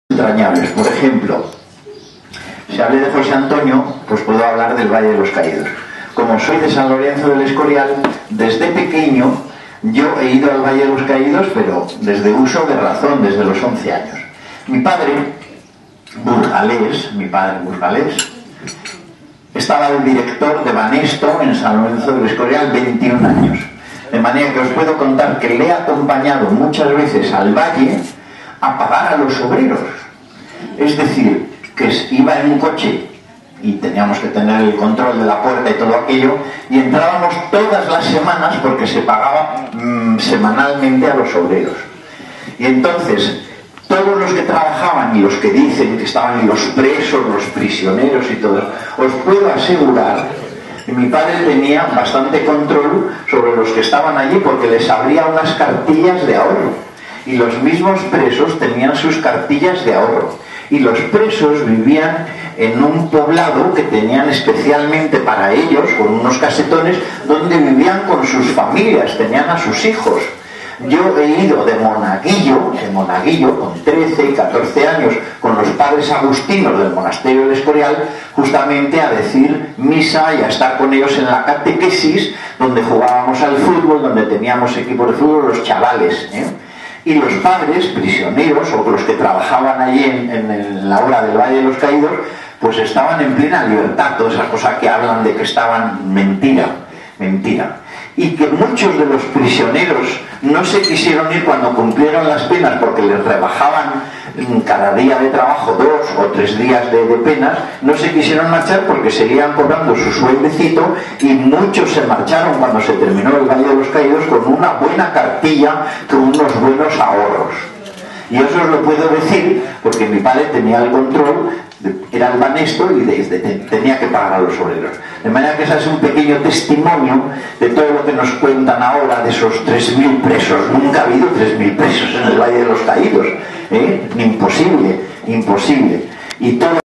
18 Julio - Bien Aparecida - Agustin Cebrian
Celebración del Alzamiento Nacional del 18 de Julio en la Bien Aparecida. Agustín Cebrián cuenta cómo era la vida de los presos y trabajadores en el Valle de los Caídos durante su construcción